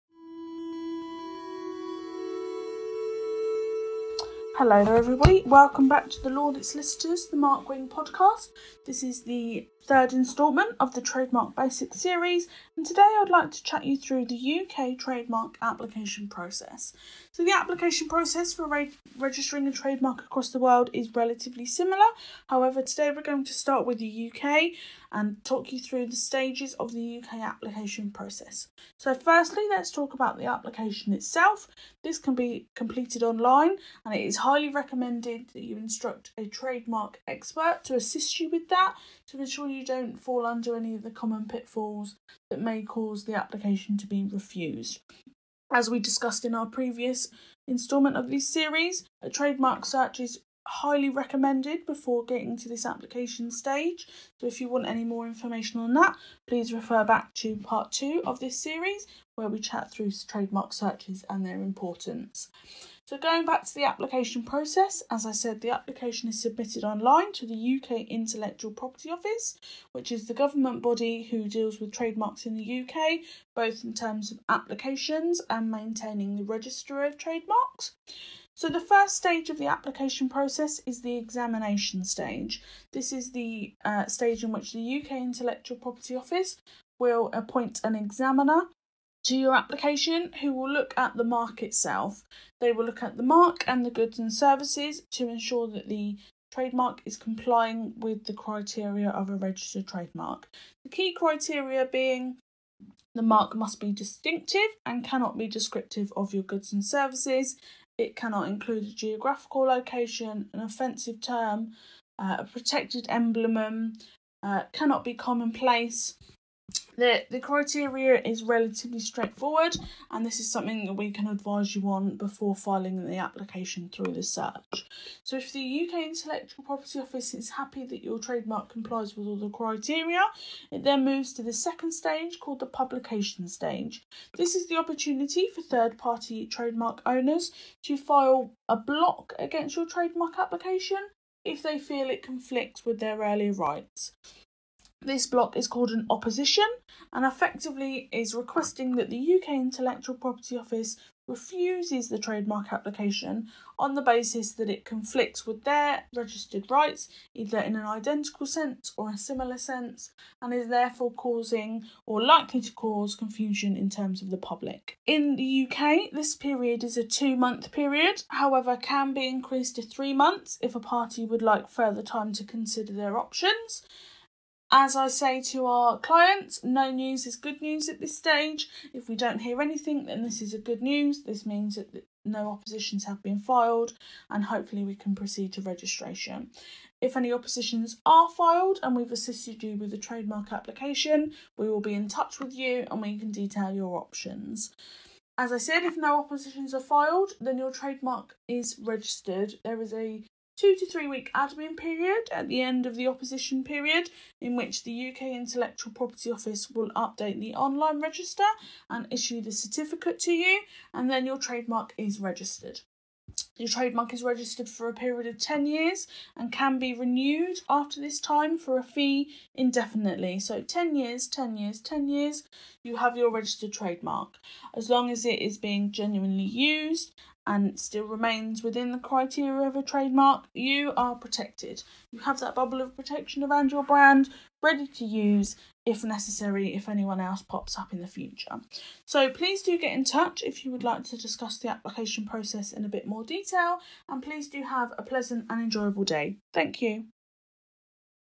The Mark Wing Podcast Ep.10 - UK Trademark application process
In the third instalment of the Trademark series Solicitor Ellis Sweetenham discusses the UK application process for Trademarks.